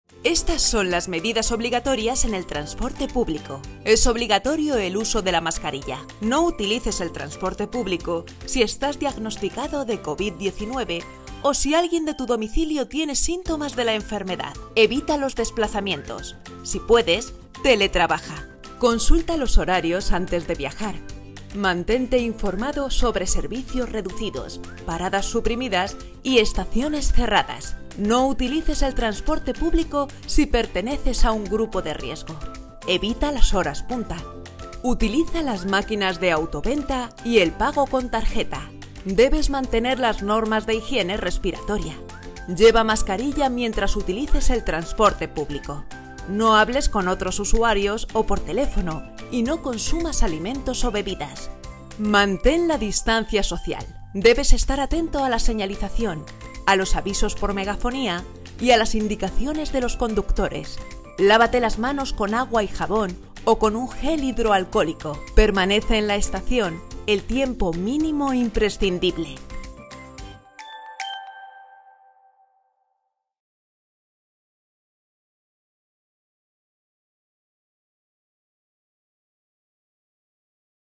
Normas para viajar en tren y otros transportes públicos durante la pandemia
Aunque estemos en la desescalada del confinamiento por el coronavirus, el virus sigue presente. Y no adoptar las medidas necesarias podría llevarnos a otro gran aumento de los contagiados y del número de muertos que podría desembocar en otro confinamiento.

Evitarlo depende de todos nosotros y de seguir las pautas que indica el Ministerio de Transportes para viajar en transporte público hasta que haya una vacuna o un tratamiento eficaz contra el SARS-CoV-2.